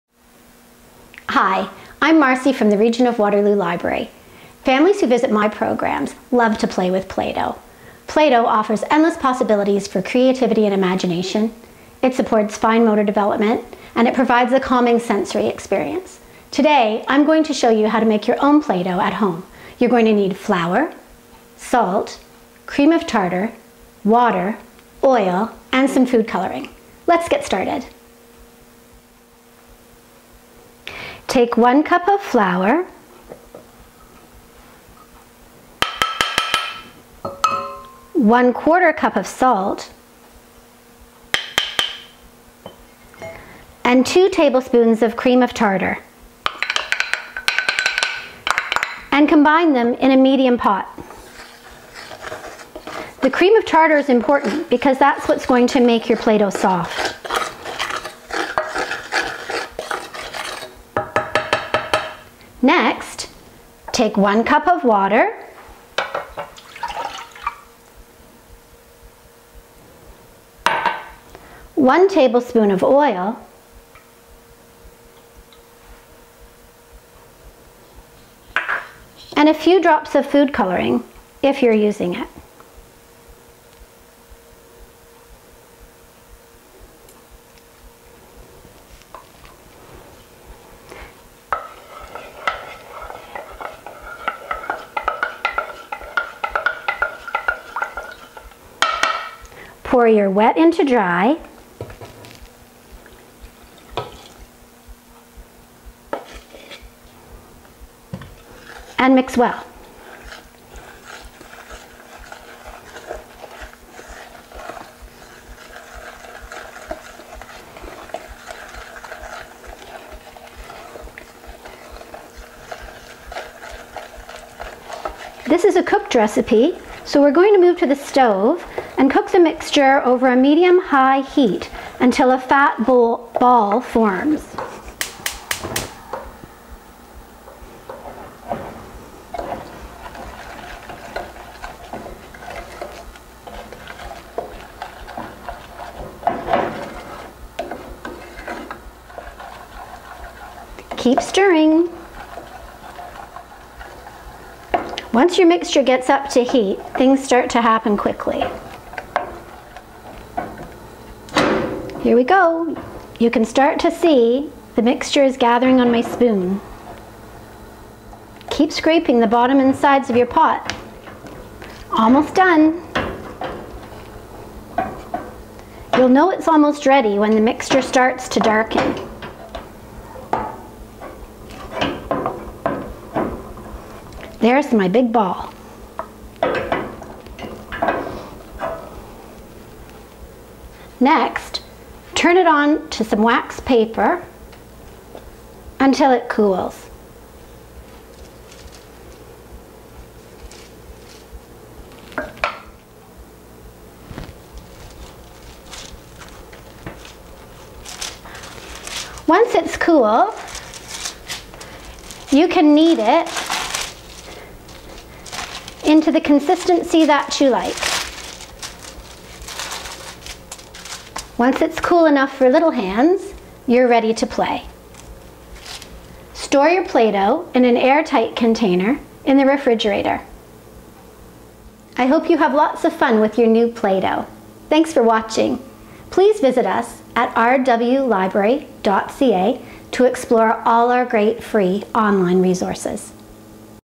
Let's Create! Making Playdough with RWL
Build marvellous creations, as RWL's Marci teaches us how to make our very own playdough, using items from the kitchen cupboard!
Supplies list:
Supplies list:
flour
salt
cream of tartar
water
oil
food colouring